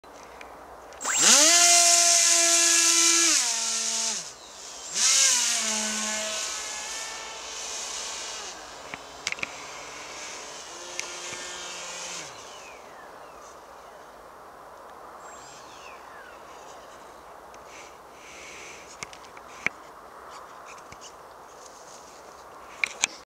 Pioneer 1400 maiden flight
Pierwszy w życiu lot samolotem RC Pioneer 1400. Nawet wylądowałem! :)